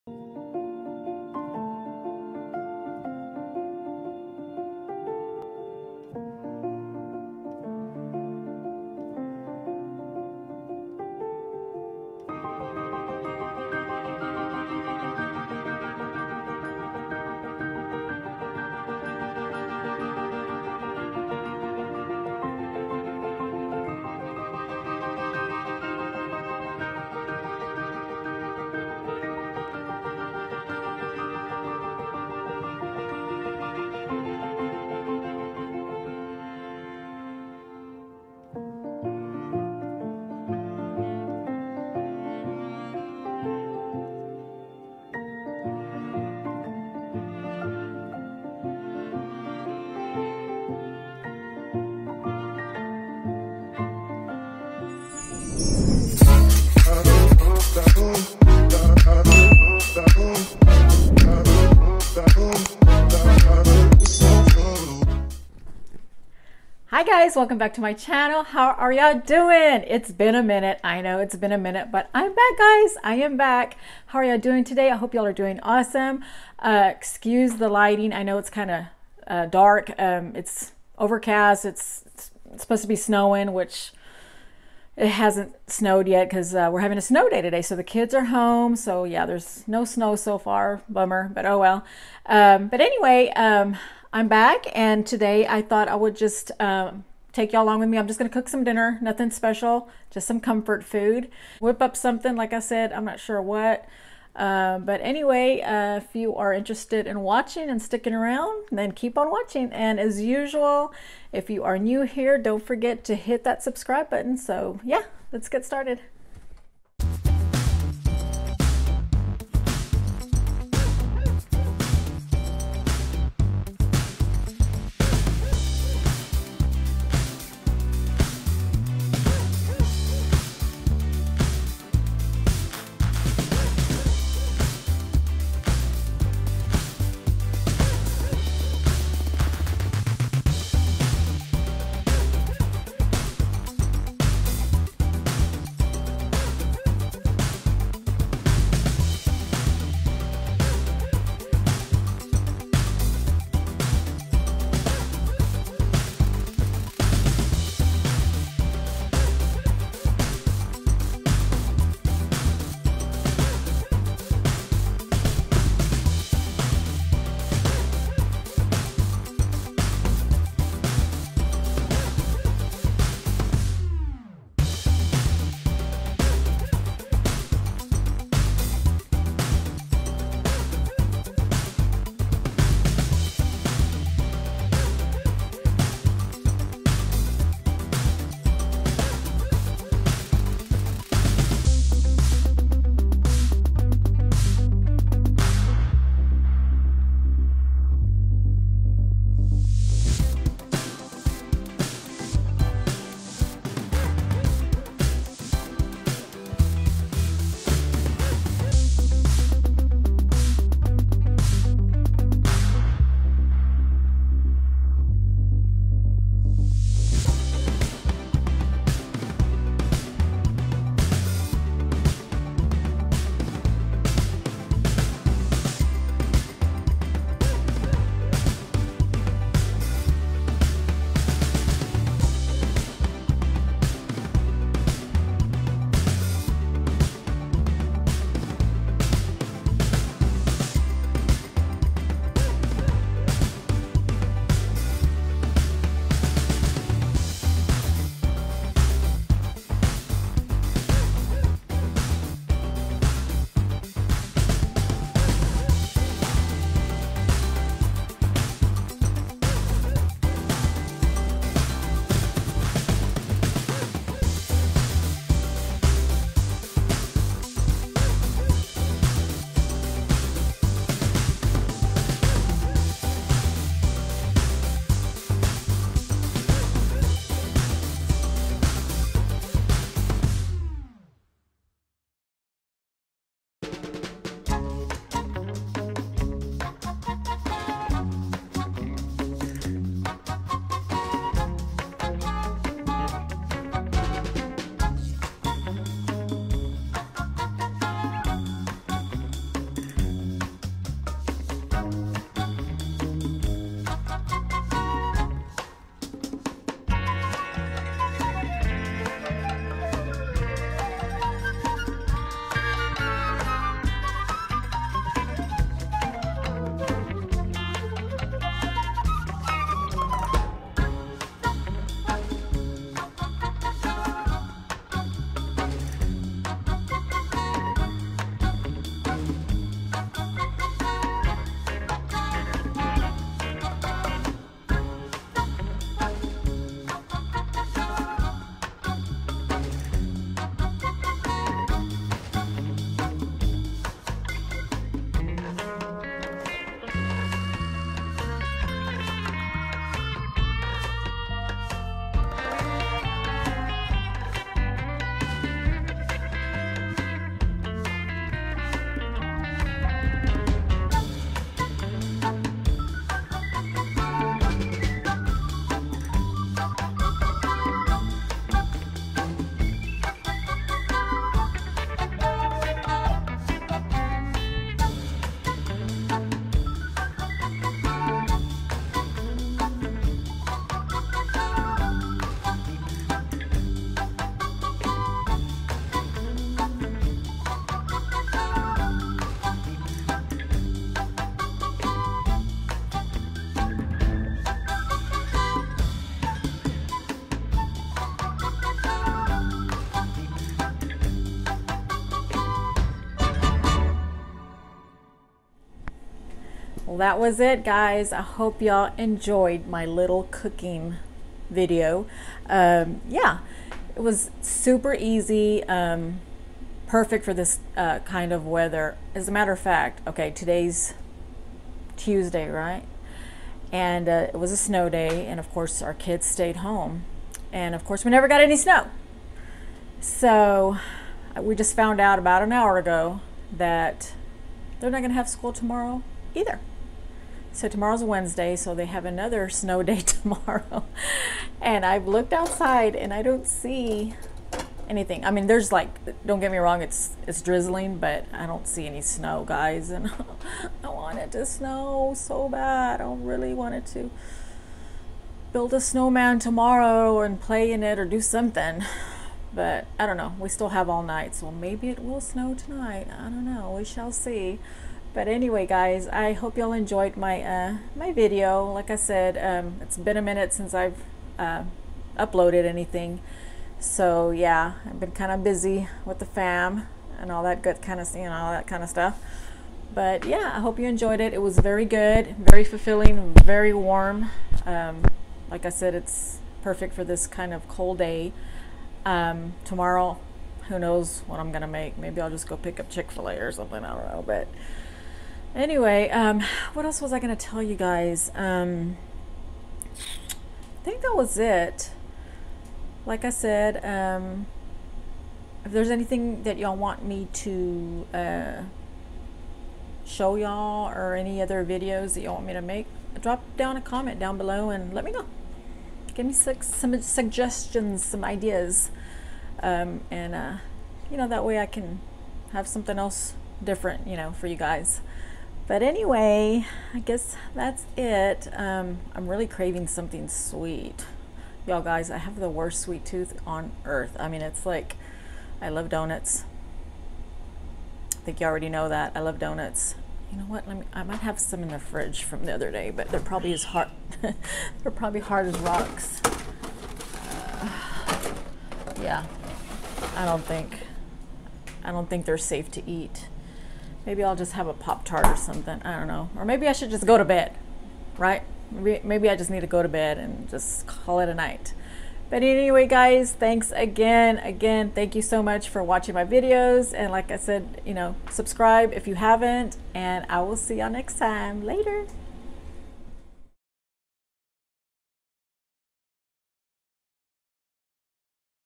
COOKING WITH ME IN 2020 (SNOW DAY) // Vogue Lyn
Today my video is about Cooking With Me In 2020 (snow day). Cooking With Me In 2020 today  happens to be a snow day in our area even though we have yet to see any snow such a bummer! Cooking with me is a perfect time Plus we just found out that there will be no school for the following day as well crazy right?!? Oh well I love when the kiddos are home plus I love cooking comfort food on cold days like this. I have another video similar to this one I’ll leave a link so you can check it out. Hope y’all enjoy!

And don’t forget to turn your notification 🛎 on so you can be notified every time I upload a new video 🤗